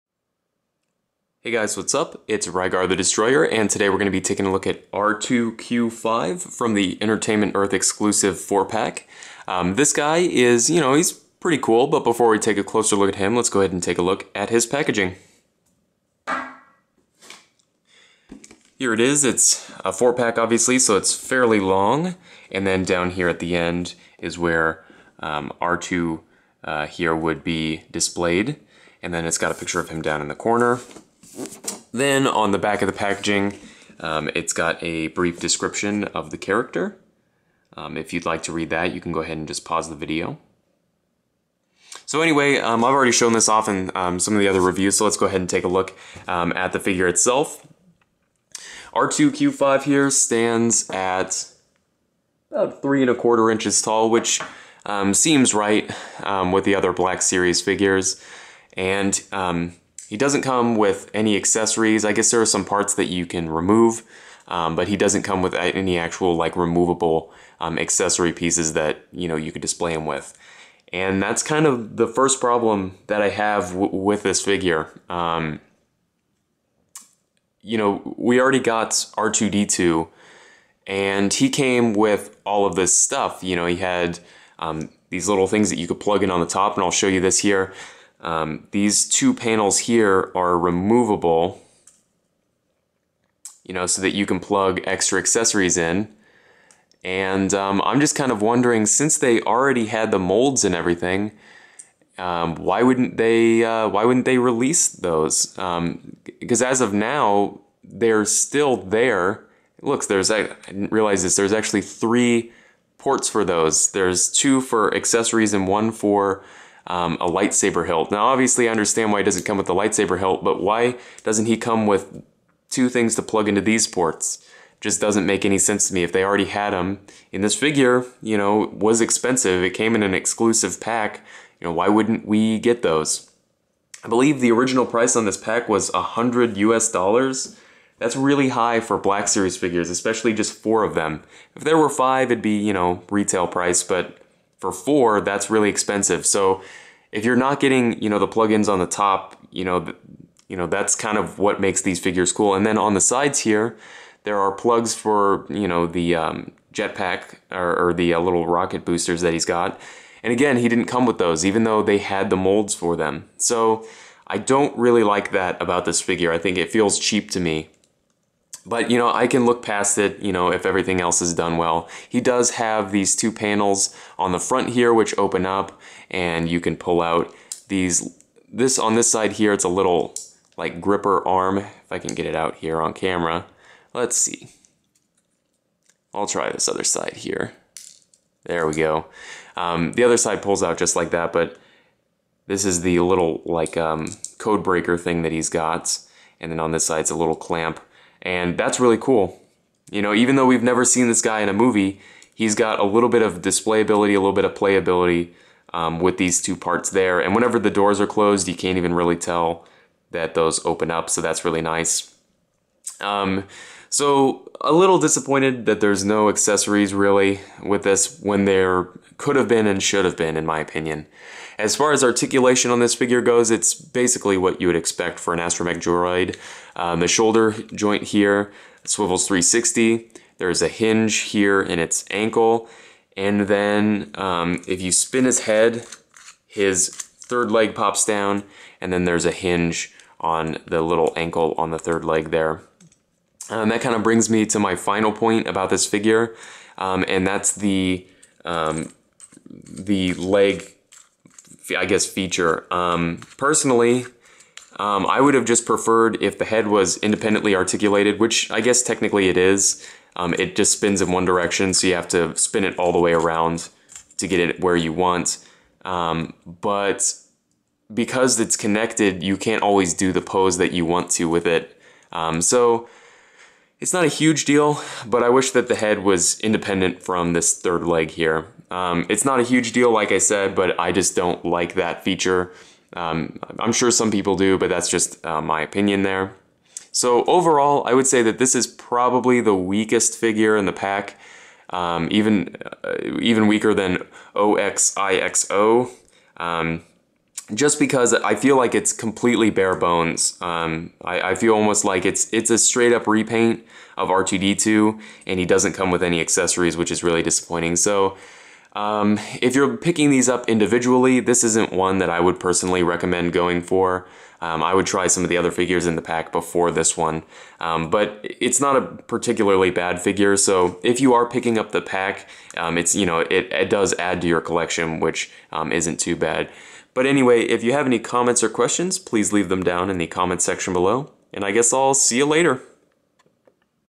Star Wars Black Series Review: R2-Q5
A quick look at R2-Q5 from the Entertainment Earth Exclusive Black Series Four Pack.